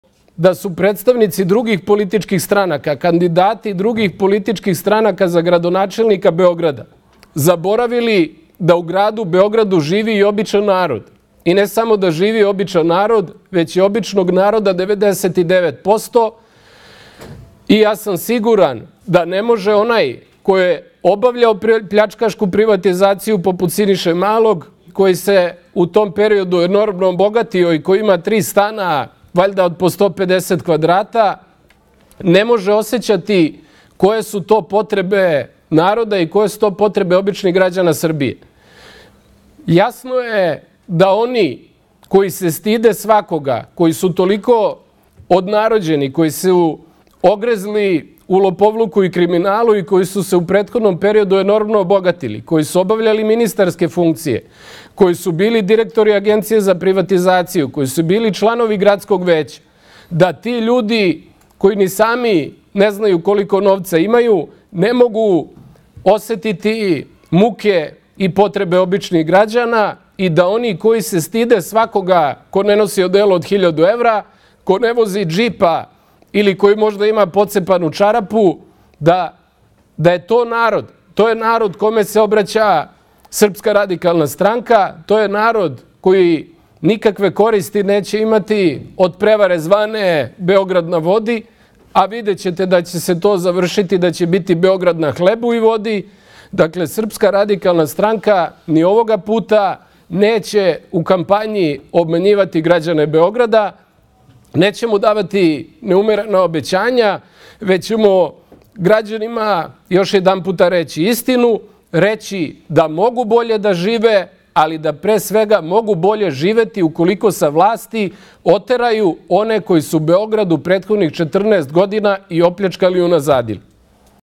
Nemanja Šarović, kandidat za gradonačelnika Beograda ispred SRS
Nemanja Šarović, kandidat za gradonačelnika Beograda, Srpske radikalne stranke, na konferenciji za novinare održanoj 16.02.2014.
SRPSKA RADIKALNA STRANKA NA INTERNETU:
sajt: